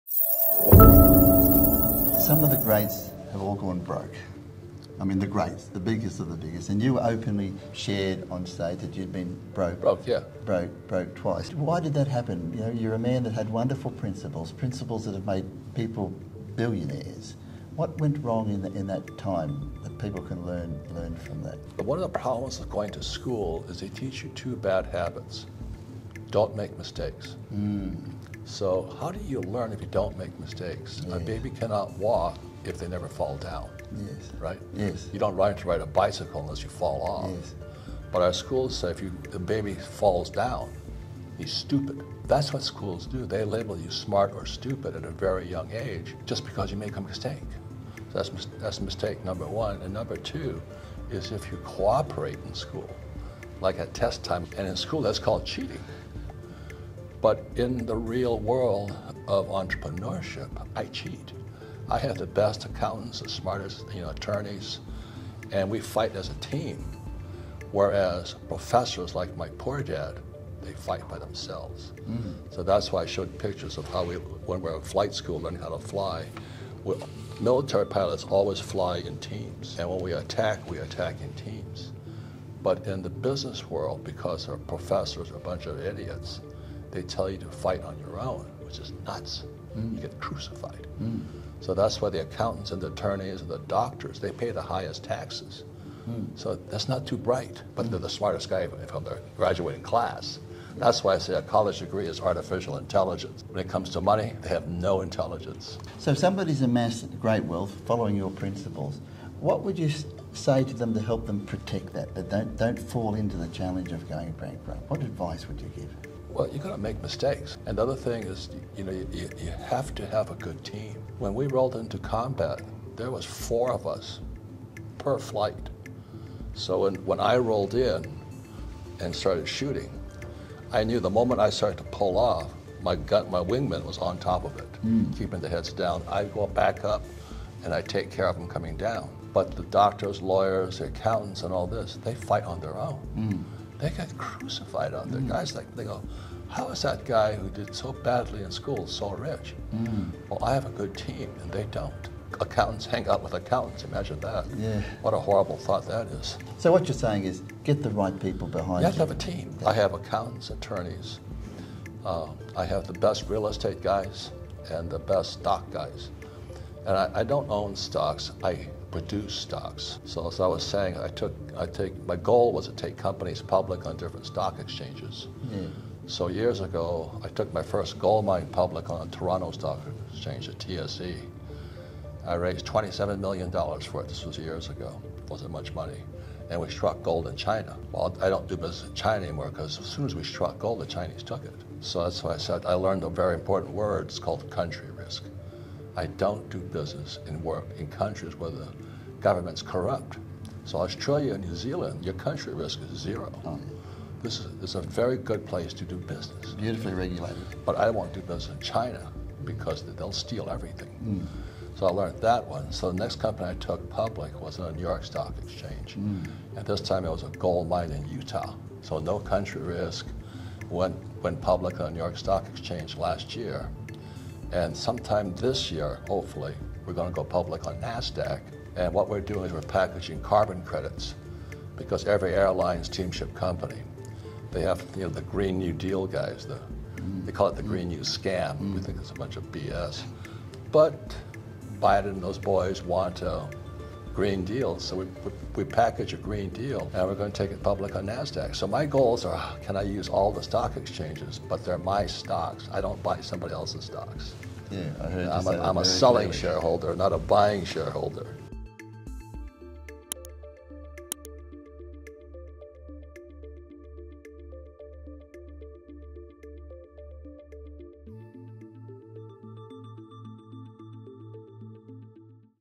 Robert Kiyosaki: My 3 Investing Tips to Build Great Wealth Fast
In this video, Robert Kiyosaki shares how he went bankrupt and what he did to recover fast and create a lot of wealth.

Robert Kiyosaki is a tycoon entrepreneur, investor, motivational speaker, author and also a financial knowledge activist. He is very popular for his book ‘Rich Dad Poor Dad’ which ever since the time of its release, scaled to become the number one personal finance book of all time.